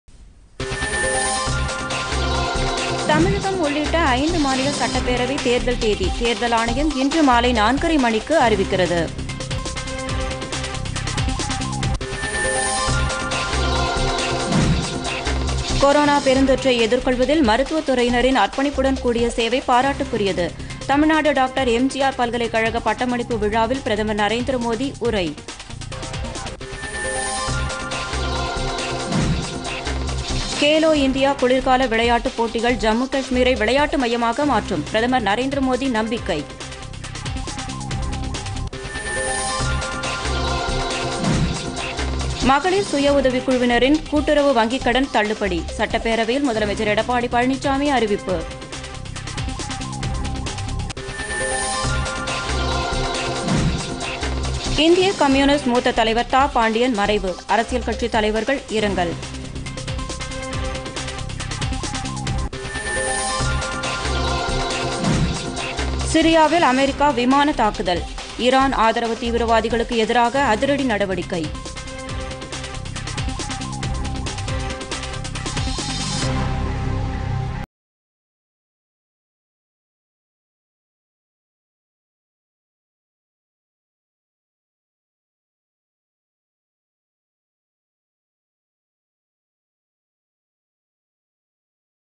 பொதிகை பிற்பகல் 2.00 மணி தலைப்புச் செய்திகள் [26.02.2021]
)    தமிழகம் உள்ளிட்ட 5 மாநில சட்டப்பேரவைத் தேர்தல் தேதி - தேர்தல் ஆணையம் இன்று மாலை நான்கரை மணிக்கு அறிவிக்கிறது

2)   கொரோனா பெருந்‍தொற்றை  எதிர்கொள்வதில்  மருத்துவத் துறையினரின் அர்ப்பணிப்புடன் கூடிய சேவை பாராட்டுக்குரியது -  தமிழ்நாடு டாக்டர் எம்ஜிஆர் பல்கலைக் கழக பட்டமளிப்பு விழாவில் பிரதமர் நரேந்திர மோதி உரை.

3) கேலோ இந்தியா  குளிர்கால விளையாட்டுப் போட்டிகள் ஜம்மு காஷ்மீரை விளையாட்டு மையமாக மாற்றும் -  பிரதமர் நரேந்திர மோதி நம்பிக்கை.

4) மகளிர் சுய உதவிக்குழுவினரின் கூட்டுறவு வங்கிக்கடன் தள்ளுபடி - சட்டப்பேரவையில் முதலமைச்சர் எடப்பாடி பழனிசாமி அறிவிப்பு.

5) இந்திய கம்யூனிஸ்ட் மூத்த தலைவர் தா.பாண்டியன் மறைவு - அரசியல் கட்சித் தலைவர்கள் இரங்கல்.

6) சிரியாவில் அமெரிக்கா விமான தாக்குதல் - ஈரான் ஆதரவு தீவிரவாதிகளுக்கு எதிராக அதிரடி நடவடிக்கை.